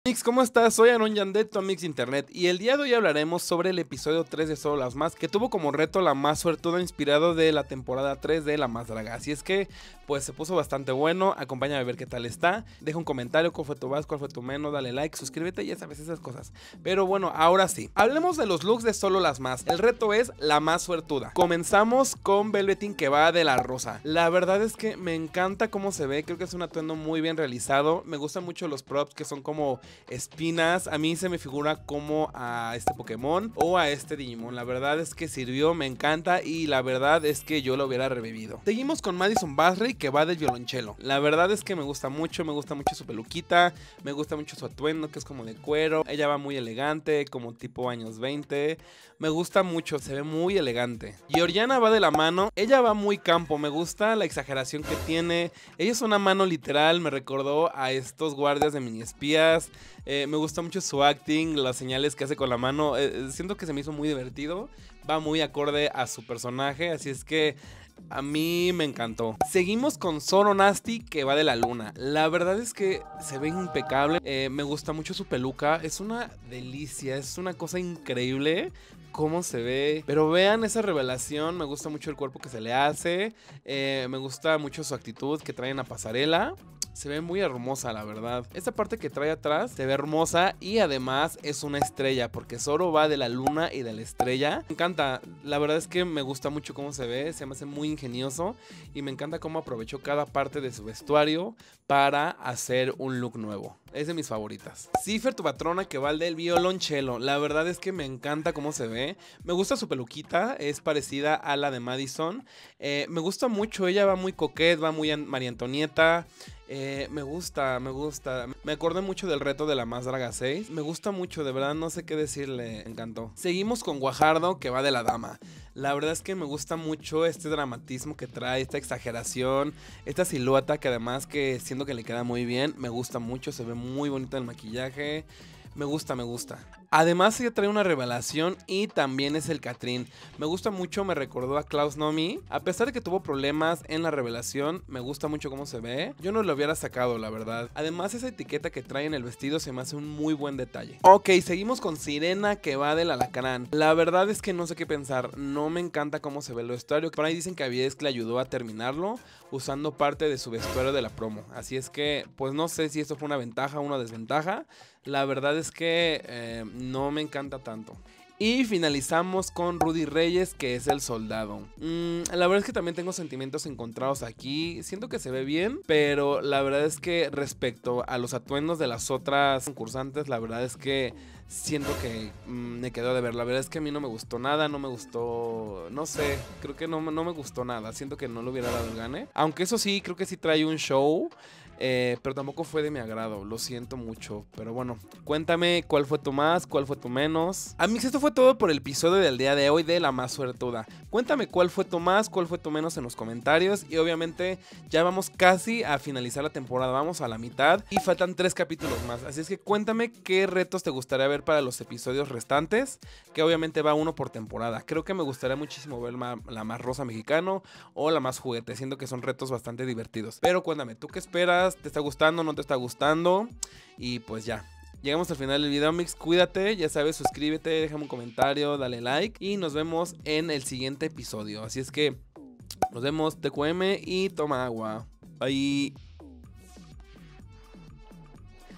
Calificando Looks La más Suertuda | La Más Draga
En esta revisión de La Más Draga hablamos de looks del tercer episodio "La más Suertuda" Cuéntanos en comentarios ¿Cuál fue tu favorito? ¿Qué te pareció el episodio?